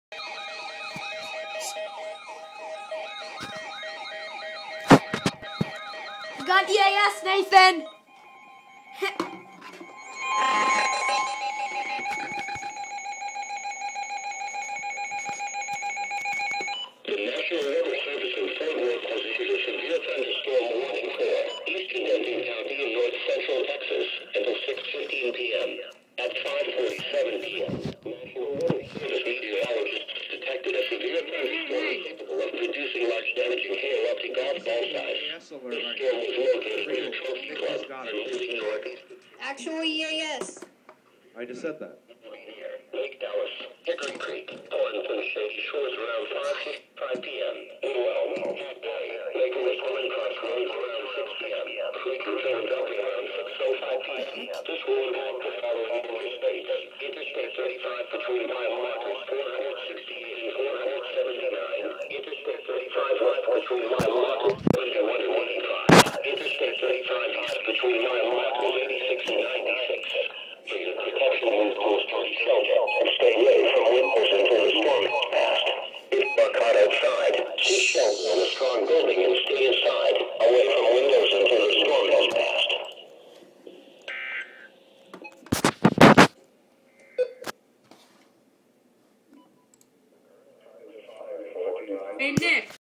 NOAA Weather Radio EAS #48: Severe T-Storm Warning
With Nick Freaknikist, Steve SuperEASFan24, Nathan NWRGeek/Elevatorsonly, & Matt SPCcranford1056.